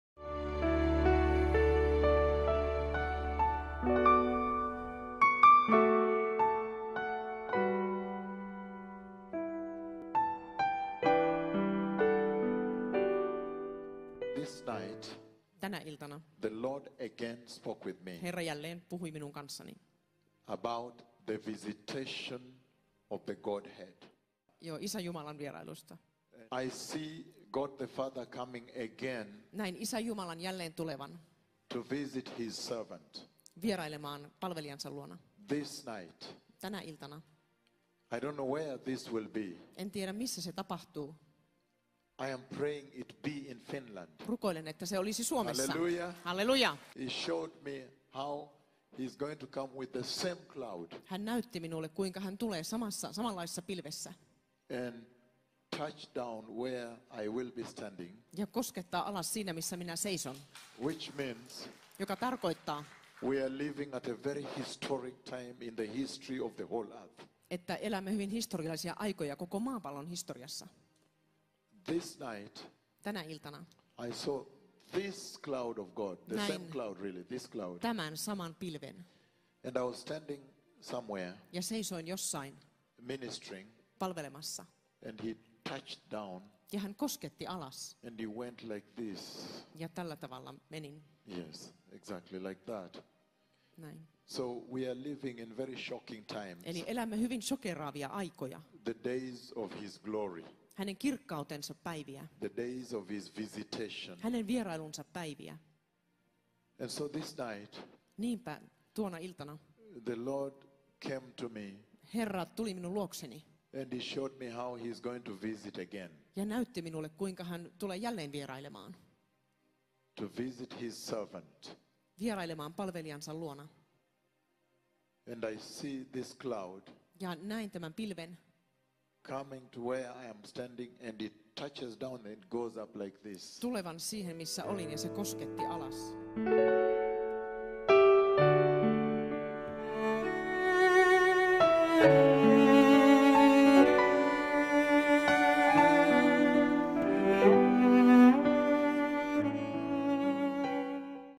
PROPHECY ON THE CLOUD OF GOD COMING TO VISIT HIS SERVANT IN A MEETING, PROPHET DR. OWUOR!
Shalom!